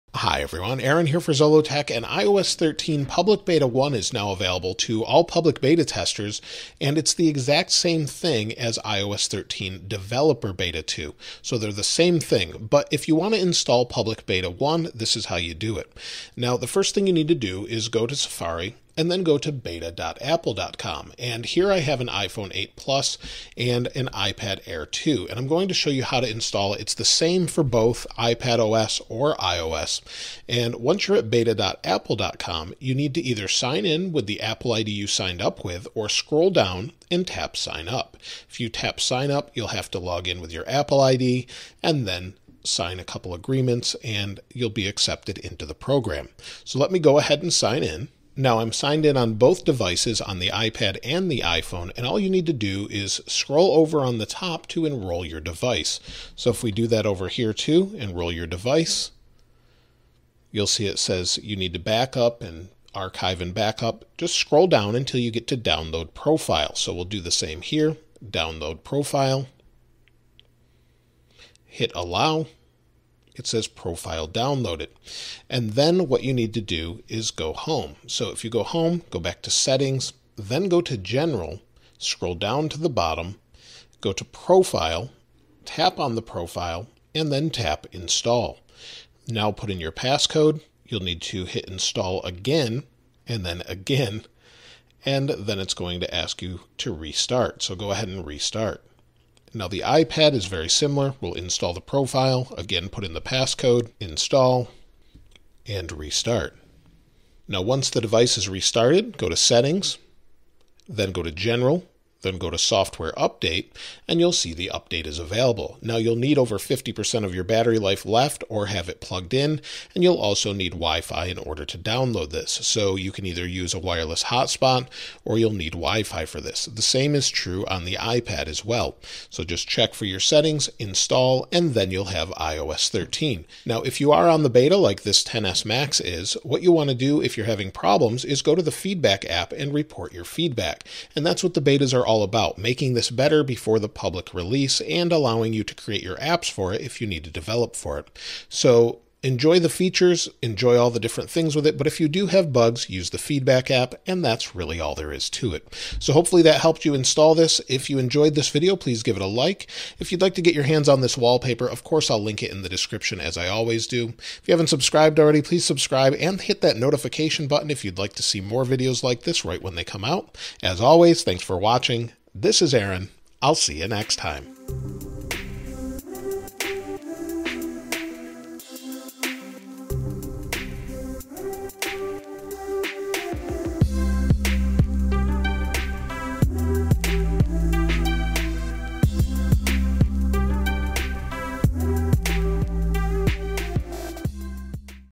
iOS 13 and iPad OS Public Betas are Out - How To Install
How to Download and Install iOS 13 Public Beta on iPhone, iPod Touch and how to install iPad OS 13 Public Beta on iPad, iPad Pro, iPad mini, and iPad Air.

iOS 13 Public Beta and iPad OS Public Beta is out and is the exact same as iOS 13 Developer Beta 2.  I show you how to install iOS 13 Public Beta 1 and iPad OS Public Beta 1 on an iPhone or an iPad.  iOS 13 Public beta is free and available to everyone worldwide.

Did you install iOS 13 Public Beta on iPhone or iPad OS on iPad?

iPhone Case I use for XS Max on Amazon
Check MacBook Prices on Amazon